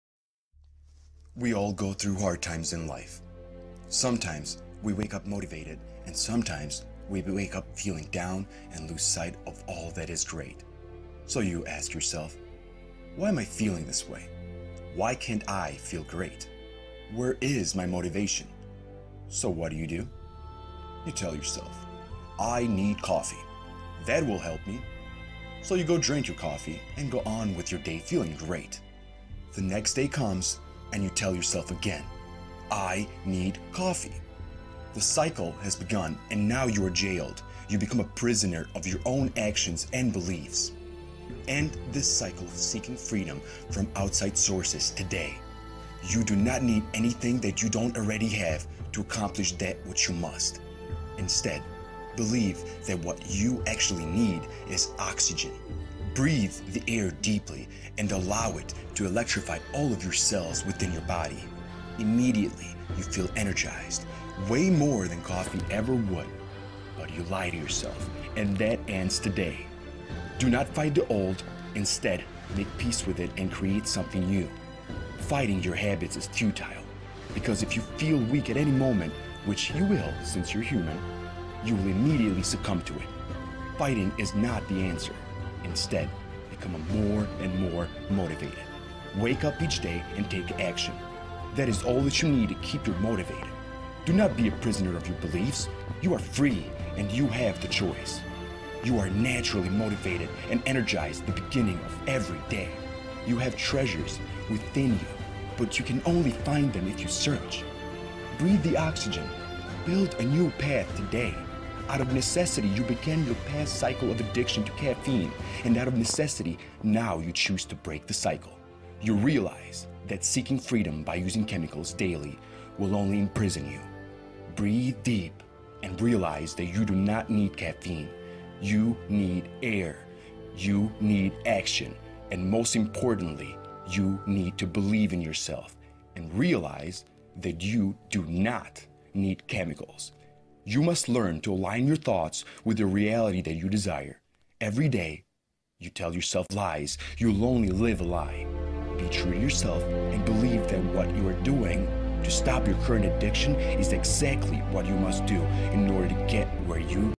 Quit caffeine TODAY!
This video will help you find the motivation to quit coffee once and for all.